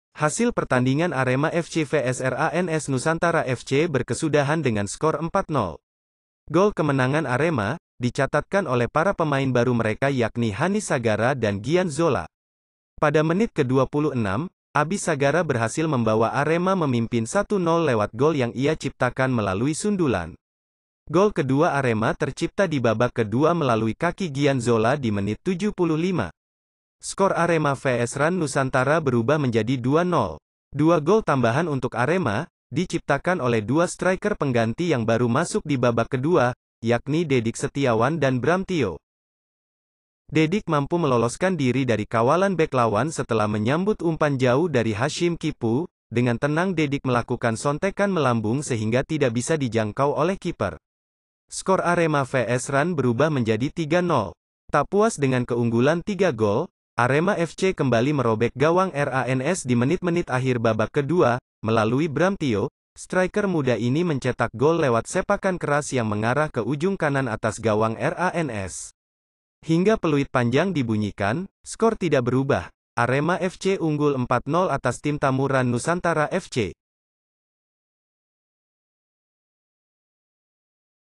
Hasil Arema FC Vs RANS FC 4-0 Laga persahabatan Hari ini
Hasil pertandingan Arema FC Vs RANS Nusantara FC hari ini Selasa 7 Juni 2022, berkesudahan dengan skor 4 0.

Gol kemenangan Arema, dicatatkan oleh para pemain baru mereka yakni Hanis Sagara dan Gian Zola.

Pada menit ke 26, Habis Sagara berhasil membawa Arema memimpin 1 0 lewat gol yang ia ciptakan melalui sundulan.

Gol kedua Arema tercipta dibabak ke 2 melalui kaki Gian Zola di menit 75. Skor Arema vs RAN Nusantara berubah menjadi 2 0.

Dua gol tambahan untuk Arema, di ciptakan oleh Dua striker pengganti yang baru masuk di babak kedua, yakni Dedik Setiawan dan Bramntio

Dedik mampu meloloskan diri dari kawalan bek lawan setelah menyambut umpan jauh dari Hasyim Kipuw,  dengan tenang Dedik melakukan sontekan melambung sehingga tidak bisa dijangkau oleh kiper. Skor Arema vs RAN berubah menjadi 3 0.

Tak puas dengan keunggulan 3 gol, Arema FC kembali merobek gawang RANS di menit menit akhir babak ke 2, melalui Bramntio, striker muda ini mencetak gol lewat sepakan keras yang mengarah ke ujung kanan atas gawang RANS.

Hingga peluit panjang dibunyikan, skor tidak berubah, Arema FC unggul 4 0 atas tim tamu RAN Nusantara FC.